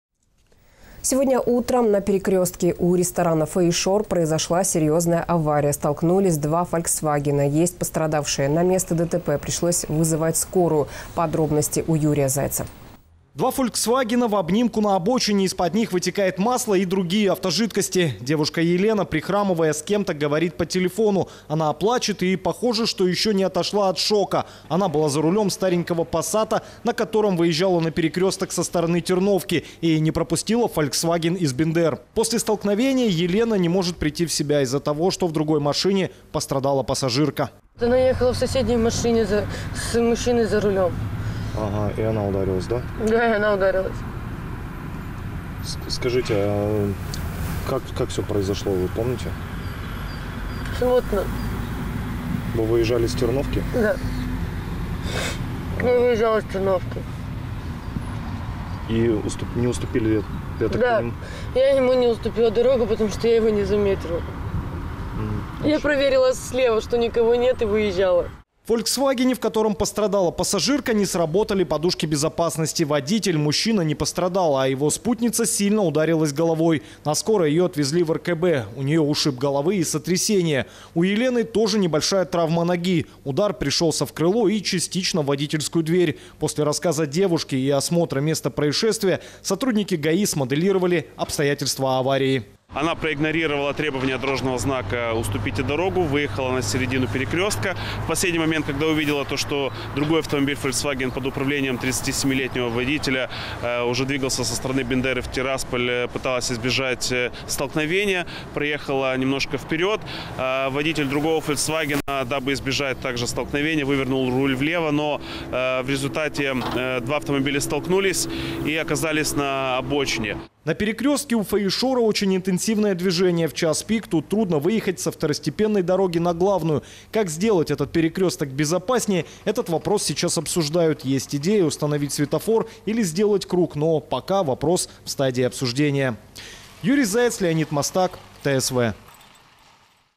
Два Фольксвагена разбились у «Фоишора»
Сегодня утром на перекрёстке у ресторана «Фоишор» произошла серьёзная авария. Столкнулись два «Фольксвагена», есть пострадавшие. На место ДТП пришлось вызывать скорую.